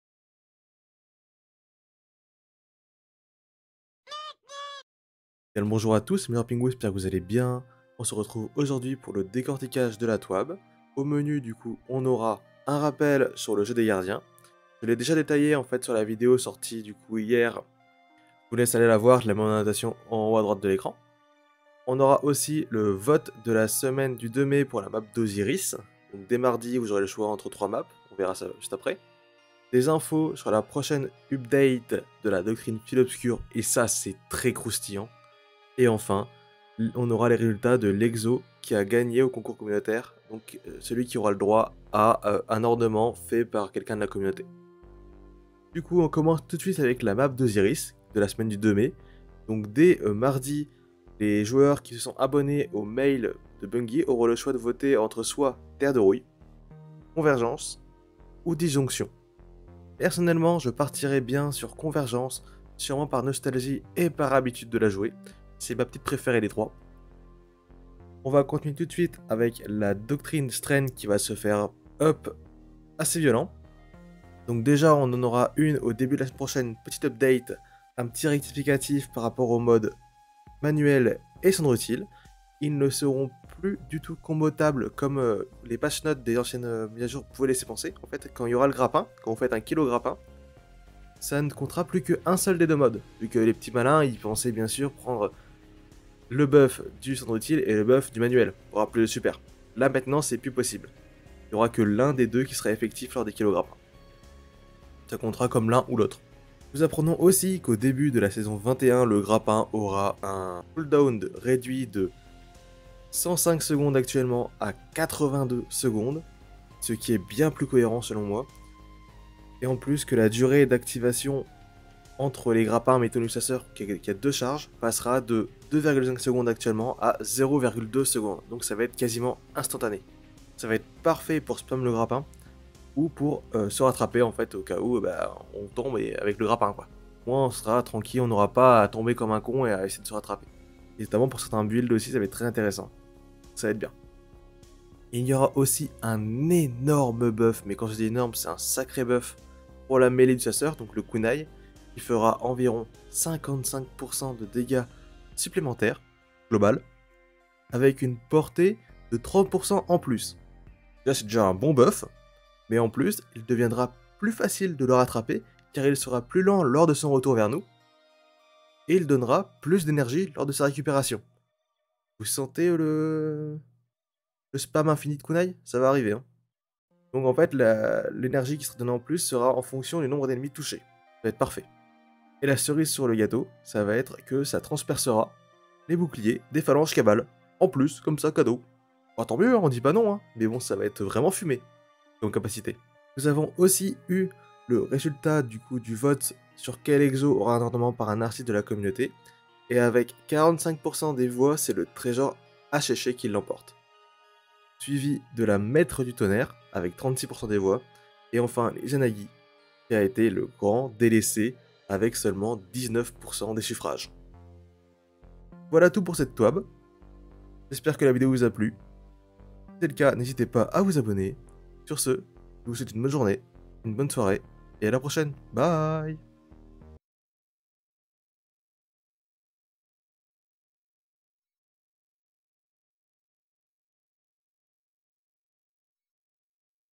Le chasseur filobscure vas devenir broken en saison 21 ! Twab 27/04/23 !
salut tout le monde , aujourd'hui c'est TWAB ! Spoiler il y a du lourd !

Prenez soin de vous , Bon courage !